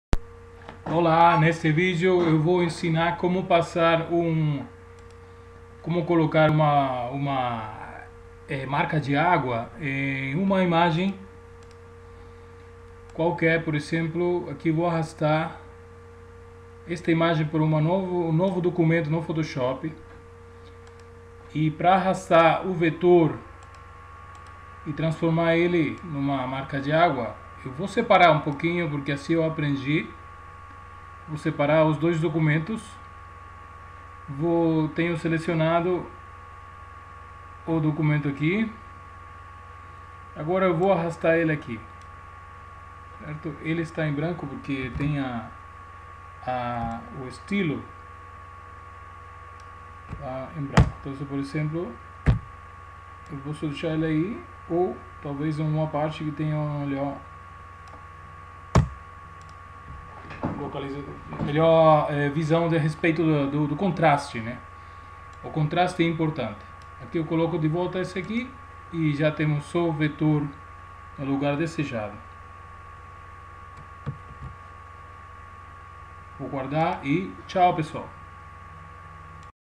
Como colocar marca d'agua vetorizada em foto Photoshop CS6
Logo de criar o logo para minha amiga fotógrafa Daiana Martins e enviar para ela o arquivo PSD, ela perguntou como colocar o novo logo nas fotos, dai nasceu esse tutorialzinho :D rsrs

DICA: Mesmo que possa ser colocada a marca d'agua com extensão PNG (com fundo transparente) sobre a imagem, é recomendável usar um vetor pois a qualidade fica maior ( o photoshop costuma alterar um pouco os PNG). Então se o seu designer lhe passou o documento "mãe" PSD ou AI (do Illustrator), onde fica guardado o vetor, aproveite!